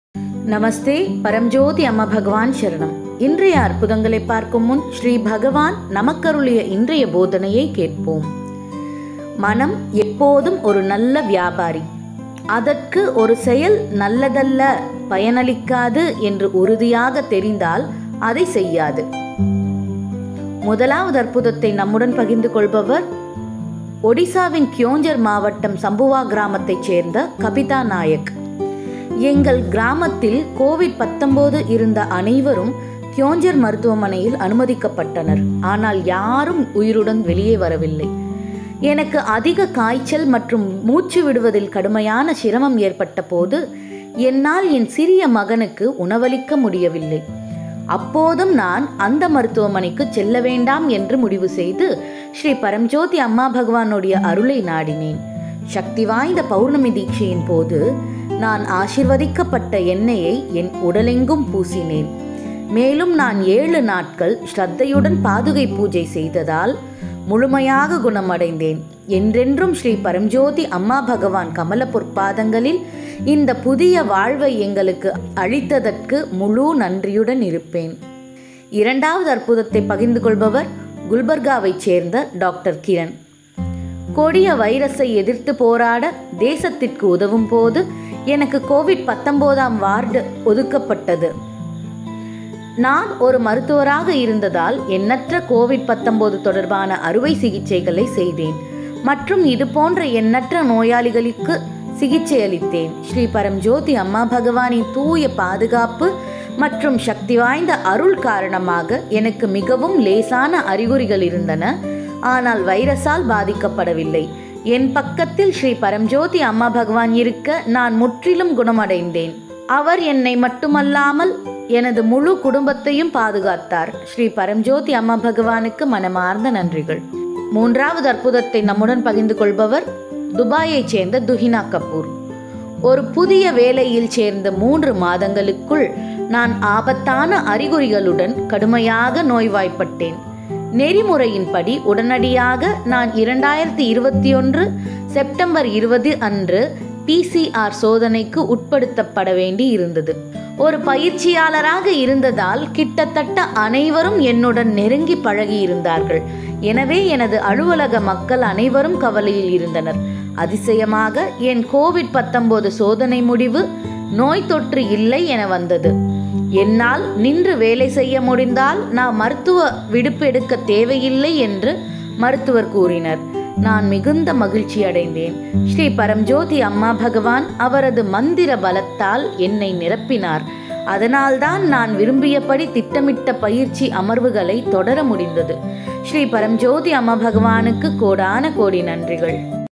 பக்தர்கள் கொரோனாவால் பாதிக்கப்படாமல் தங்கள் கடவுளால் காப்பாற்றப்பட்டார்கள்
𝗽𝗿𝗼𝗴𝗿𝗮𝗺 𝗱𝗲𝘁𝗮𝗶𝗹𝘀

ஸ்ரீ பரம்ஜோதி அம்மா பகவான் ஆலயதரிசனம்